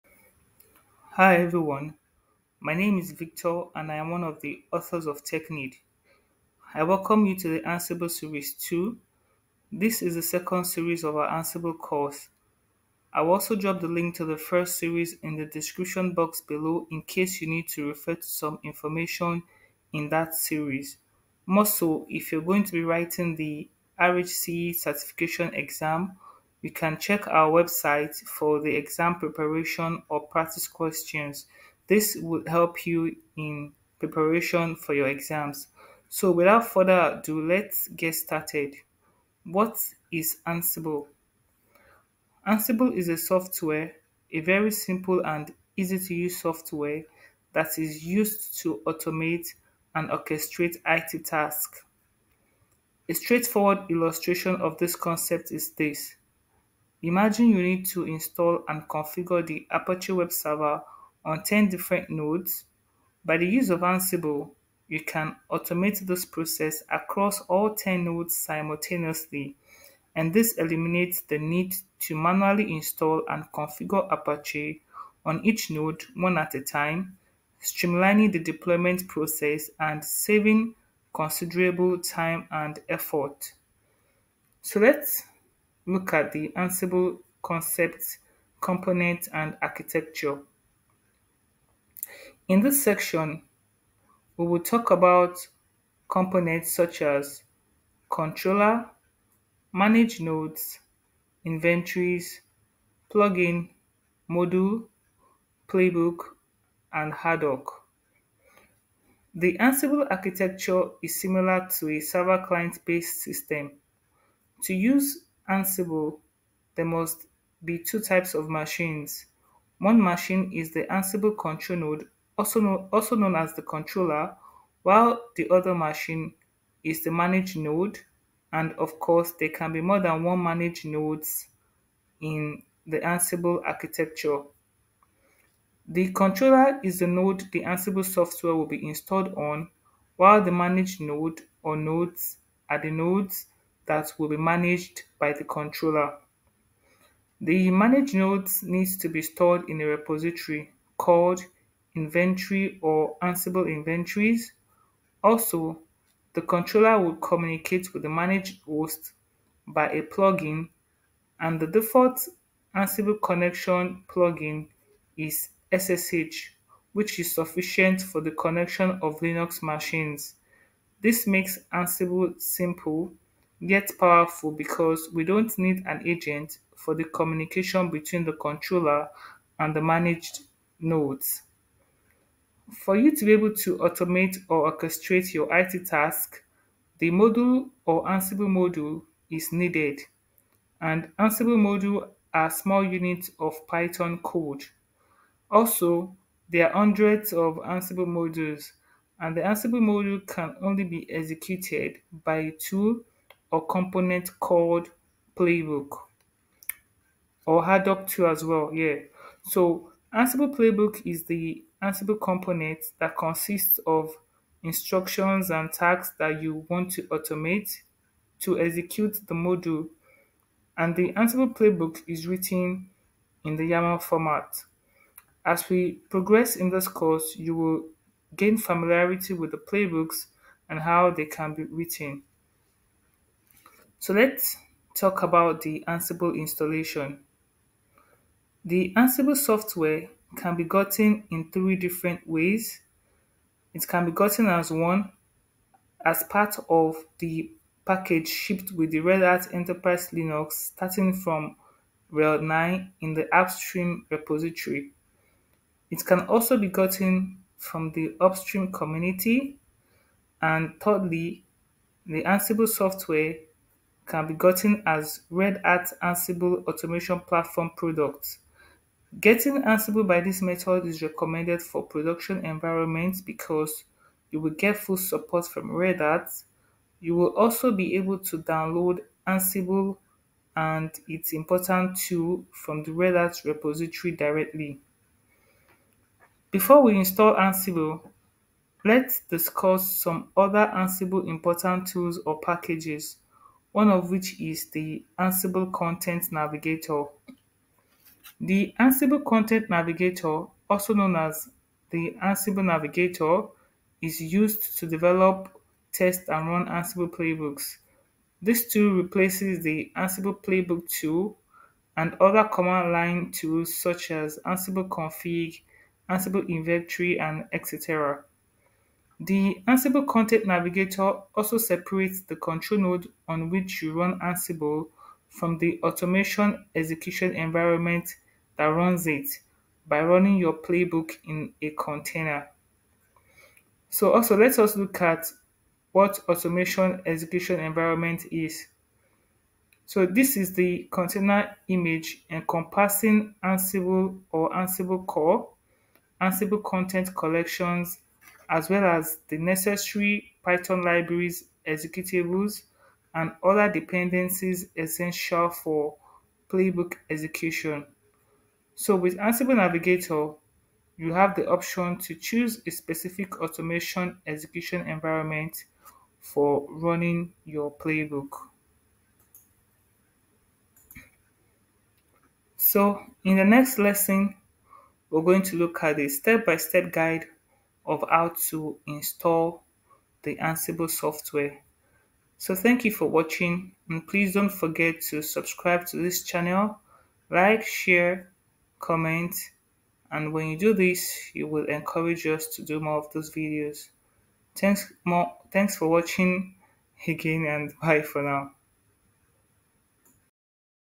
Introduction To Ansible Series 2 - Ansible Components & Architecture - Lesson 1 | RHCE 9 | ex294
In this Lesson, you will understand what Ansible is, Ansible concept, compnents & Ansible Architecture. You will also understand the different methods to install Ansible based on RHEL 9